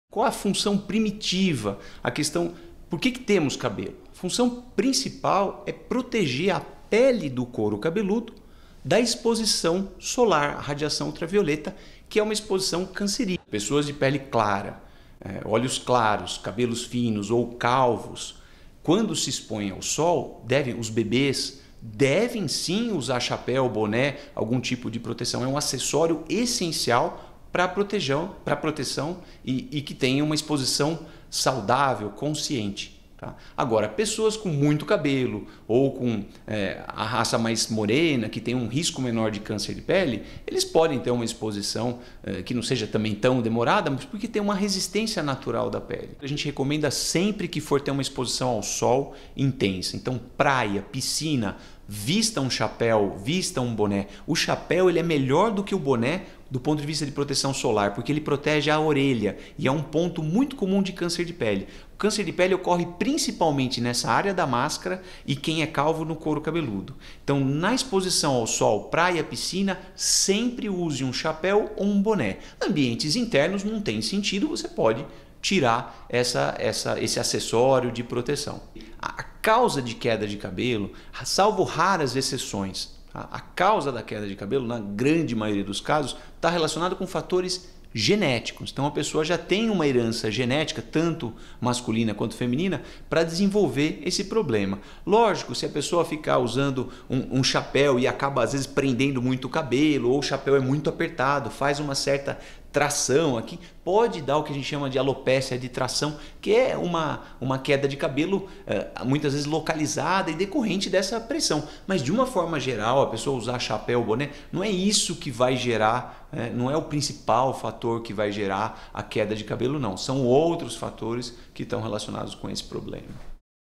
PARA QUE SERVE O CABELO?
Qual a função principal do nosso cabelo?
E o uso de chapéus ou bonés, quando são necessários?
Assista esse vídeo e entenda mais sobre uma importante parte do nosso corpo.
Fique à vontade para comentar e compartilhar!
Na dúvida, procure sempre seu médico dermatologista.

Produção: TV TEC Jundiaí e Luiz Gameiro

LUIZ GAMEIRO
CRM/SP 102134 - RQE 24960
Médico Dermatologista - Jundiaí/SP